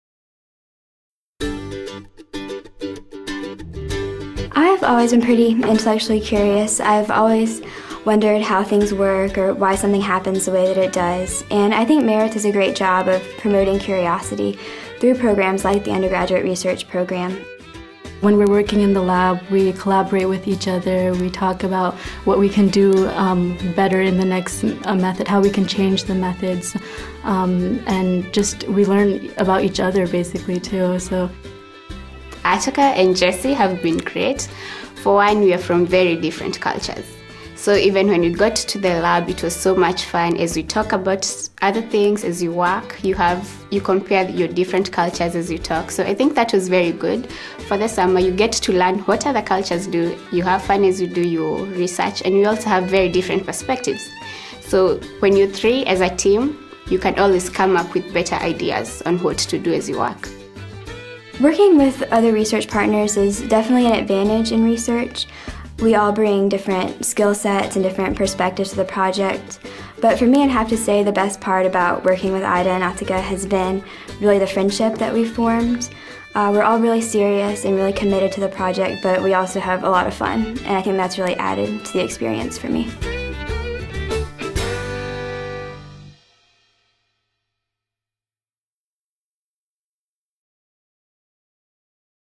Intellectual Curiosity : Meredith College
Meredith works hard to instill a sense of intellectual curiosity in our students—not simply because we are expected to as an educational institution, but for the sheer joy of it. Through programs like undergraduate research, Celebrating Student Achievement day, and the Honors Program, Meredith students become well-versed in the research process, from developing an appropriate question to presenting their research at academic conferences. Ida Githu, '13, Aatiqah Khalid, '12, and Jessie Breazeale, '12, are collaborating on a research project focused on identifying possible HIV protease inhibitors found in a Kenyan plant—working together, they are enjoying the opportunity to both hone their research skills and deepen their friendships.